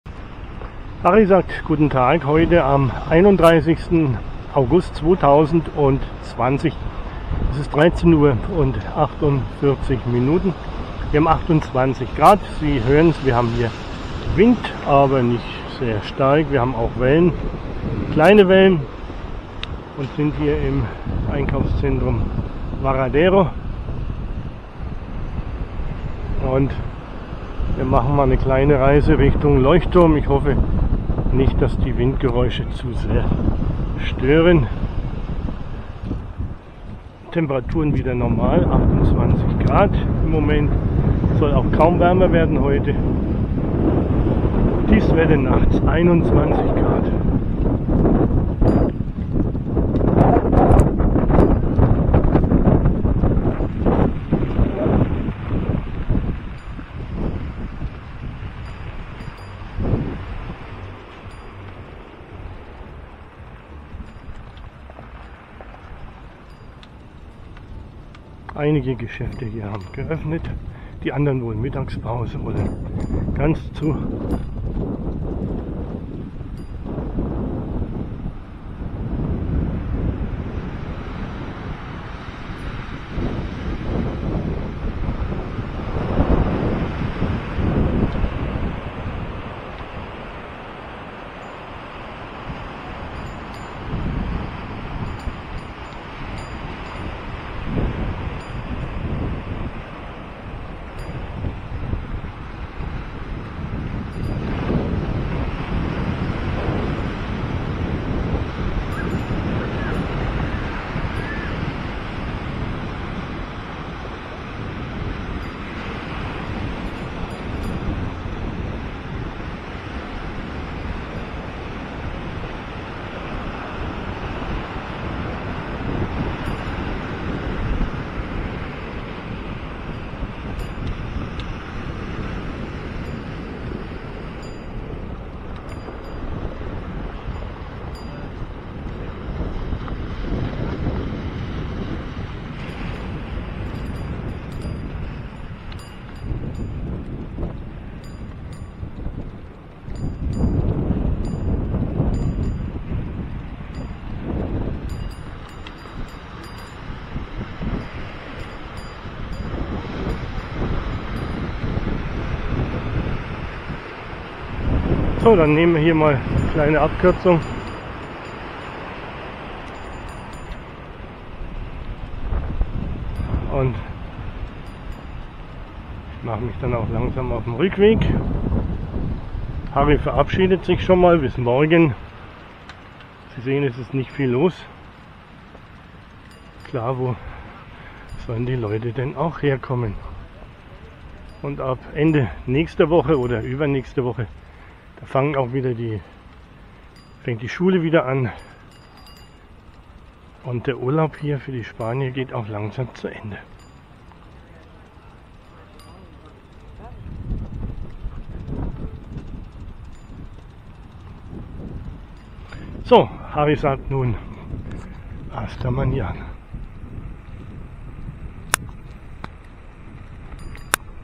Gran Canaria Maspalomas Wettervideo 31.8.20
...28 Grad. etwas Wind. keine Wellen. kein Wolken. um 13.30